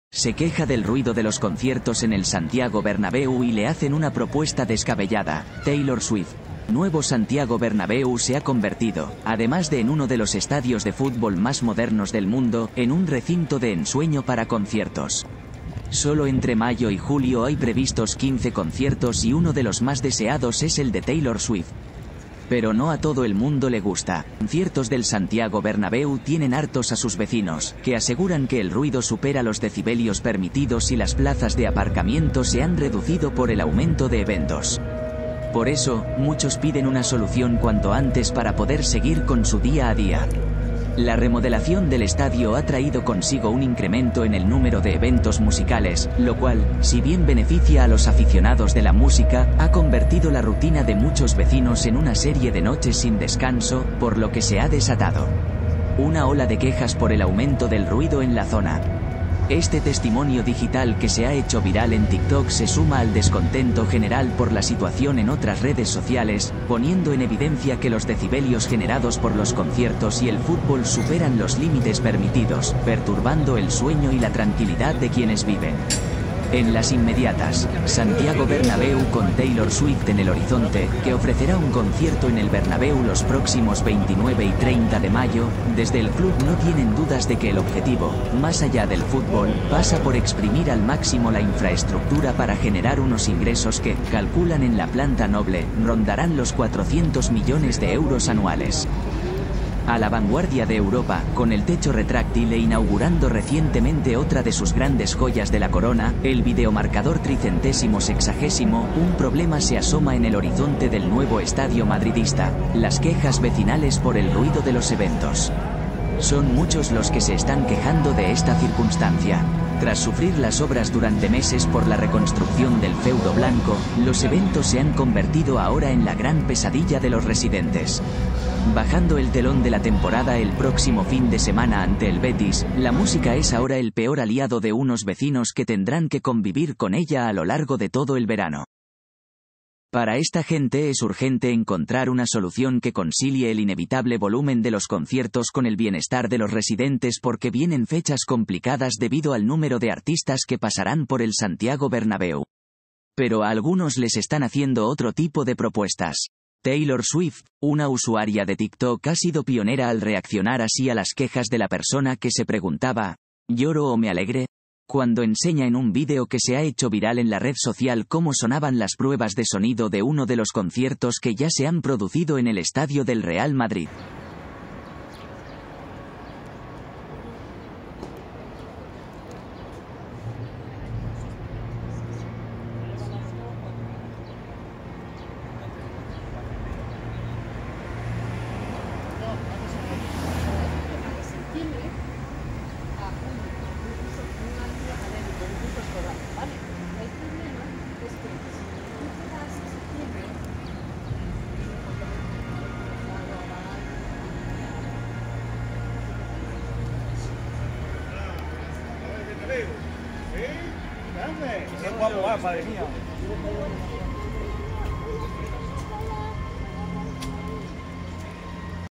ESTADIO SANTIAGO Bernabéu Futbol sí música no, los vecinos del Bernabéu, molestos por los conciertos
Los conciertos del Santiago Bernabéu tienen hartos a sus vecinos, que aseguran que el ruido supera los decibelios permitidos.Y las plazas de aparcamiento se han reducido por el aumento de eventos. En tan solo dos meses hay previstos 15 conciertos por lo que piden una solución cuanto antes para poder seguir con su día a día., Amin Madrid